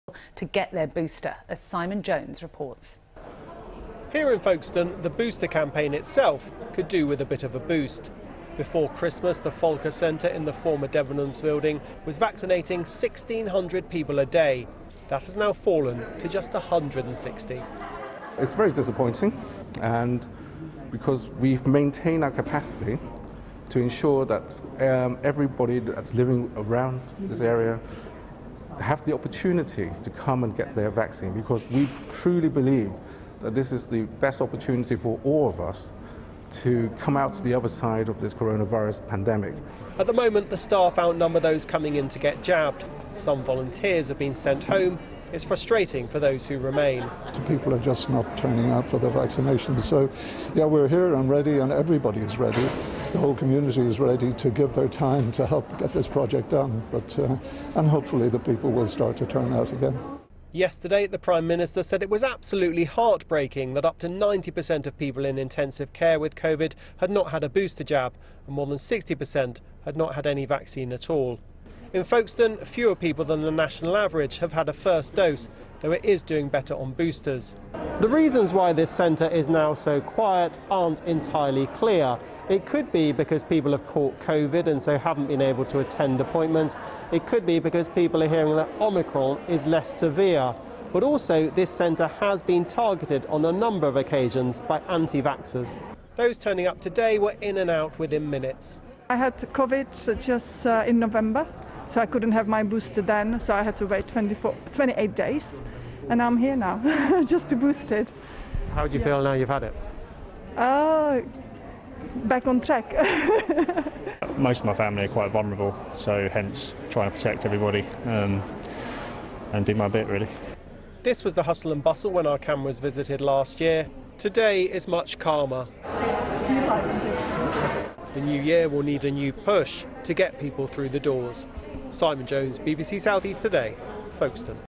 Channel Rotary Folkestone Vaccination Booster Campaign
The number of people getting jabbed at the Folkestone Vaccination Centre is lower than expected and needed.  We are seeking your help, through your various TV, radio, social etc channels, to encourage people to urgently get vaccinated.
Joe Sullivan  of Channel Rotary who on behalf of the two local Rotary Clubs,  has led the recruitment and management of 700 volunteer marshals to enable the NHS to perform nearly 200,000 vaccinations here.